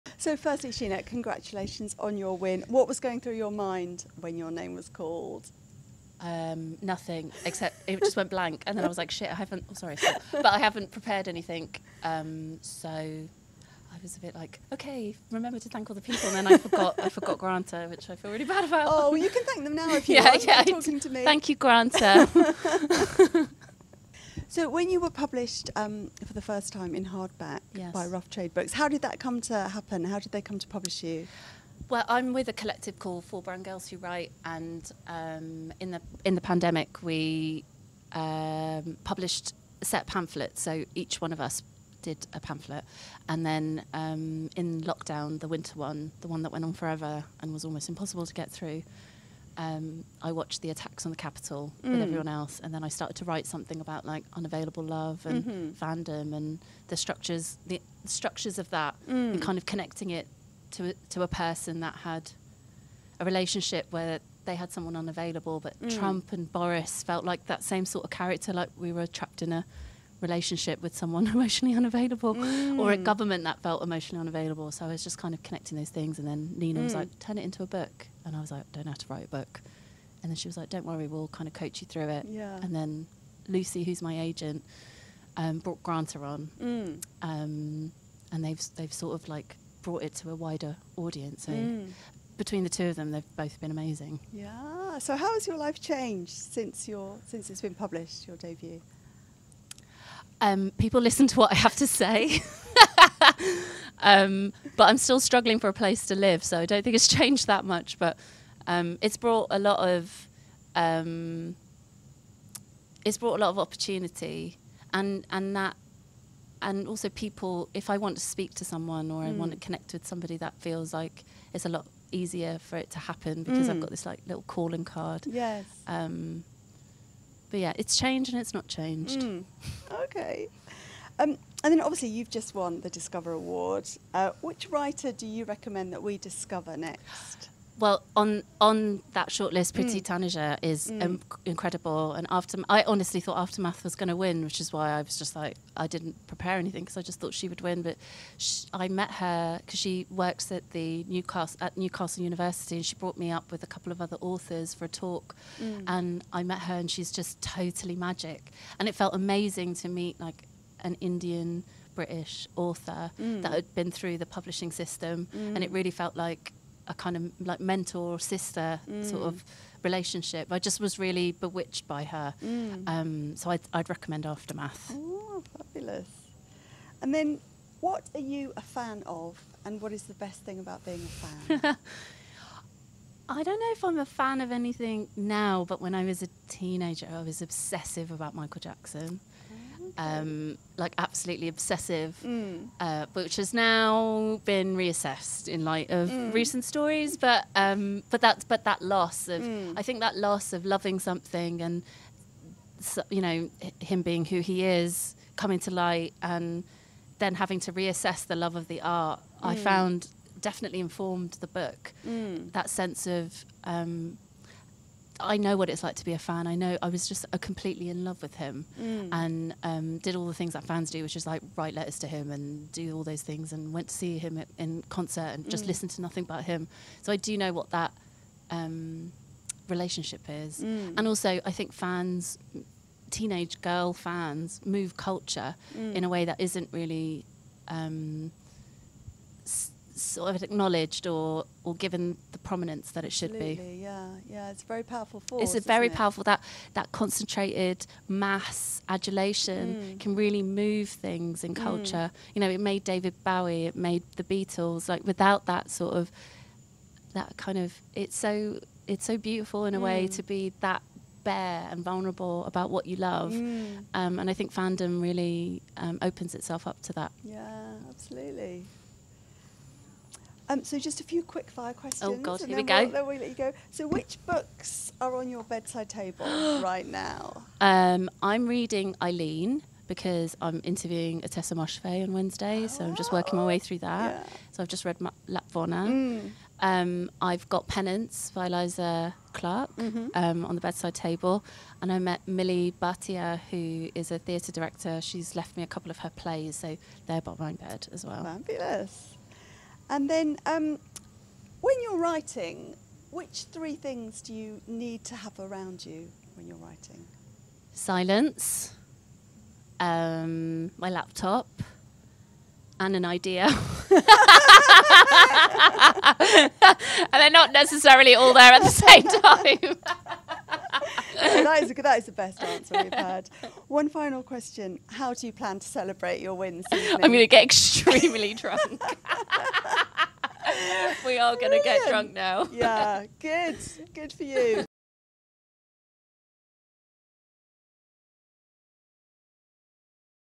Sheena Patel Interview - Nibbies Studio - The British Book Awards 2023
Sheena Patel joins us in the studio after winning Book of the Year - Discover at The British Book Awards.